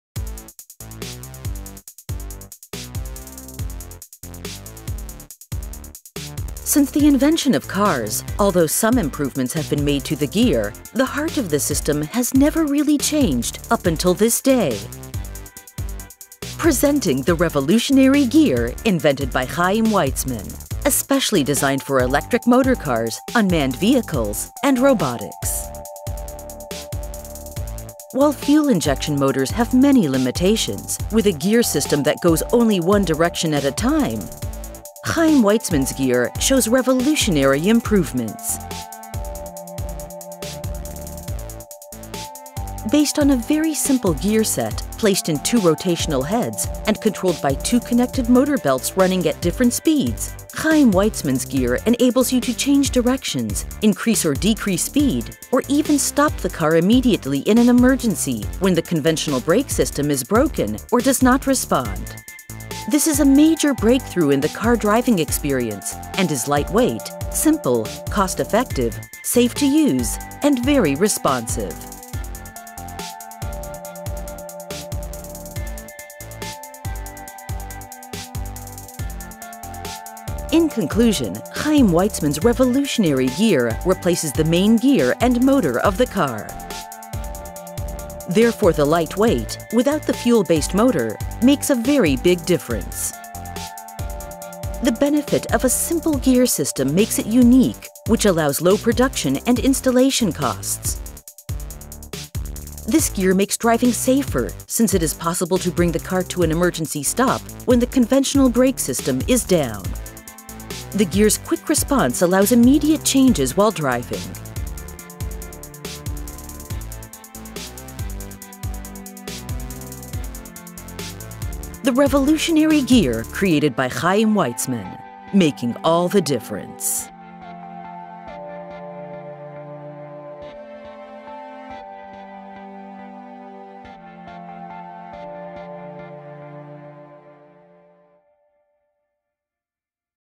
alio3d-Animation - The Revolutionary Gear of Haim Weizman
גיר חדשני ומהפכני של חיים וייצמן, מיועד למכוניות, מנועים חשמליים ולרובוטיקה.

בחסות קבוצת באראט - Barat Group

קריינות:
Victoria Feinerman
Professional US English Voiceovers

אנימציה ועריכת וידאו:
Andy Henik: alio3d - Animation & Video Editing